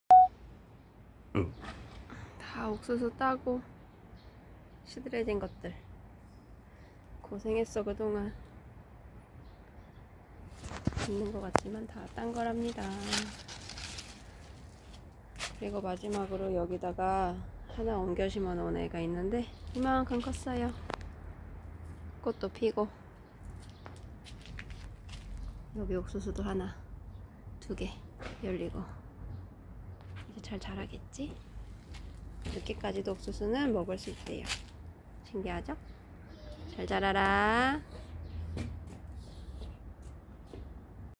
옥상텃밭 옥수수 이야기: 곁가지가 자라나 옥수수나무로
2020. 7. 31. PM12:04

옥수수 곁가지가 꽤나 커서
화분 하나 만들어 옮겨주었다.

비가 오랫동안 많이 내리고
옥수수는 크게 자라났다.

벌써 두개나.
보랏빛 수염이 보인다.
자리만 잡아주면
역시 자연이 키우는구나.

곁가지 몇개 옮겨주어야 할 것들이 있는데
비도 많이 오고
나도 이제 슬슬 귀찮은 마음이...

끝까지 성실하게 하는 사람에게
맛있는 열매가 있으리라.

난 일단 좀 쉴게 ㅋㅋㅋㅋ